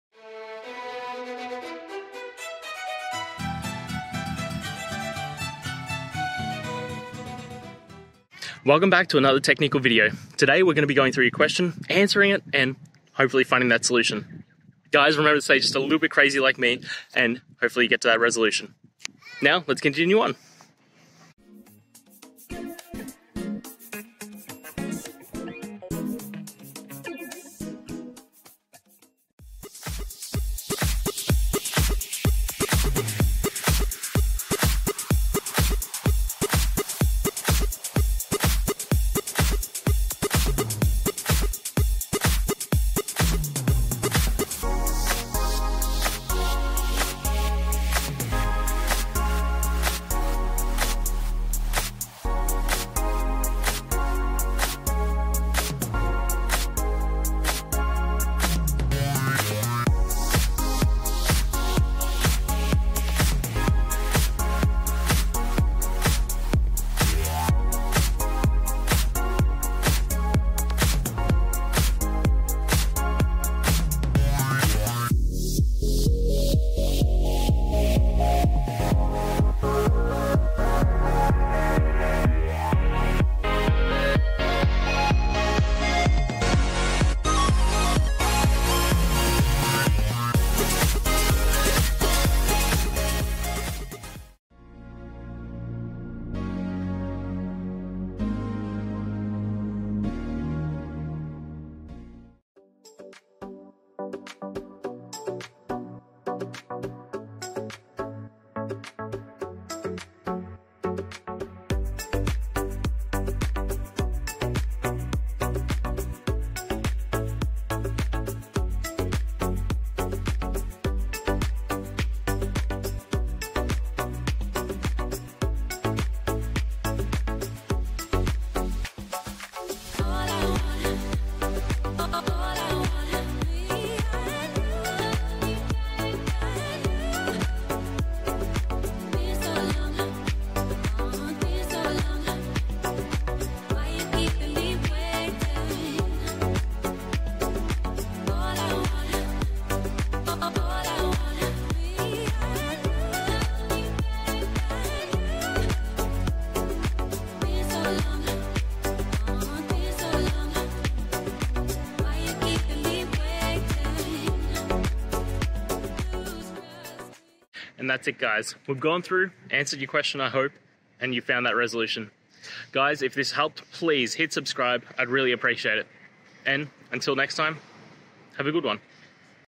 Fixing Error 1: `make` Failed When Installing sqlsrv on PHP - Troubleshooting Guide
In this video, we tackle a common issue faced by developers when trying to install the sqlsrv extension for PHP: the dreaded "make failed" error. Whether you're setting up a new environment or updating an existing one, this troubleshooting guide will walk you through the steps to diagnose and resolve this error effectively. Join us as we break down the process and provide practical solutions to get your SQL Server connection up and running smoothly.

Today's Topic: Fixing Error 1: `make` Failed When Installing sqlsrv on PHP - Troubleshooting Guide

Related to